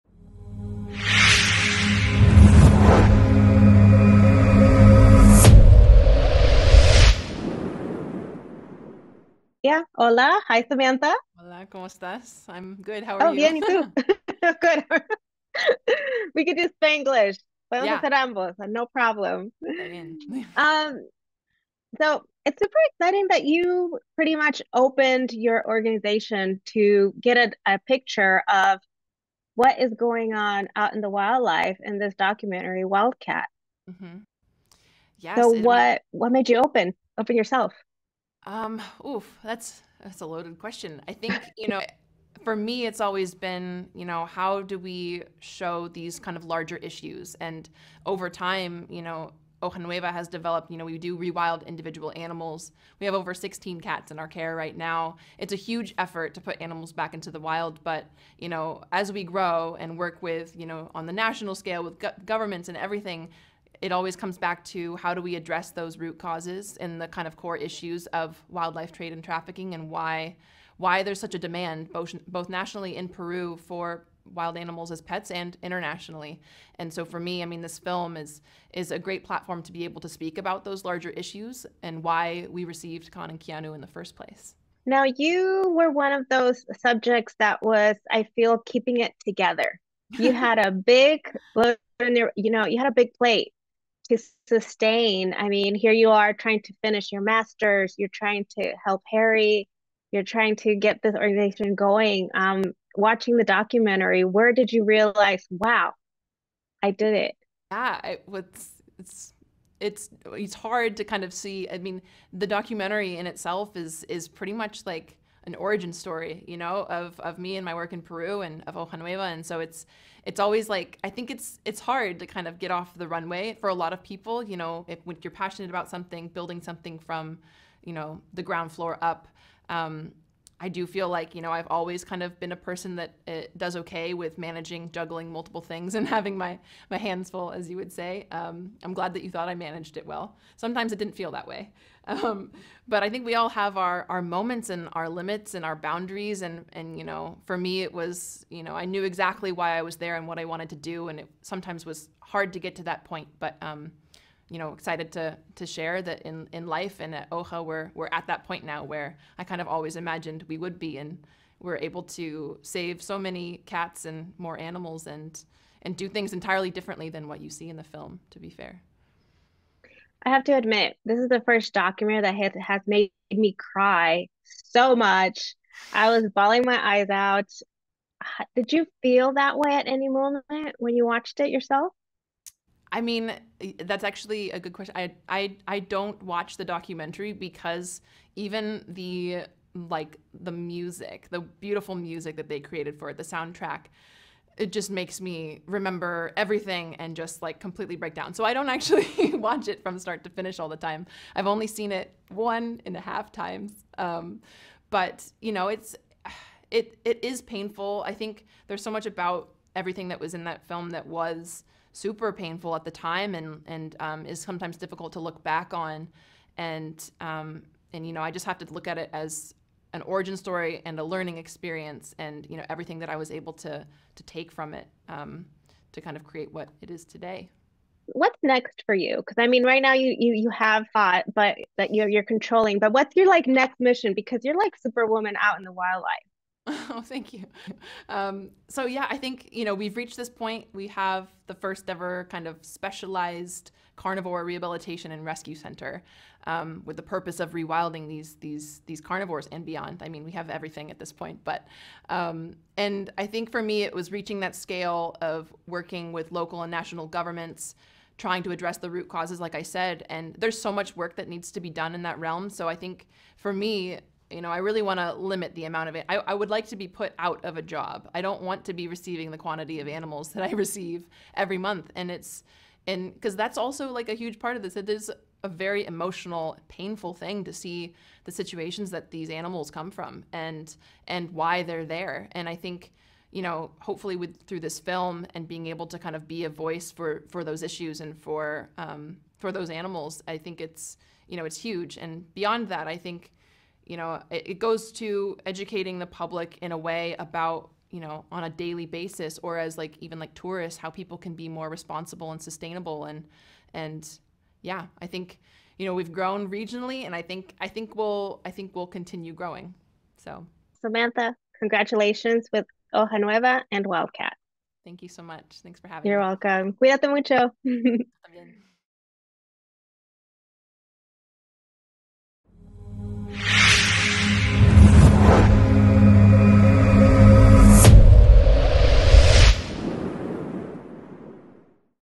Samantha Zwicker Opened Up About Her Experience In Watching Wildcat
LRM Online's (Latino Review Media) Nancy Tapia interviewed Samantha Zwicker for Wildcat documentary.

Wildcat follows the emotional and inspiring story of a young veteran (Harry Turner) on his journey into the Amazon. Once there, he meets a young woman (Samantha Zwicker) running a wildlife rescue and rehabilitation center, and his life finds new meaning as he is entrusted with the life of an orphaned baby ocelot. What was meant to be an attempt to escape from life turns out to be an unexpected journey of love, discovery, and healing.

Film subjects Harry Turner and Samantha Zwicker
Directors/Producers Melissa Lesh and Trevor Beck Frost

Wildcat is available globally on Prime Video December 30, 2022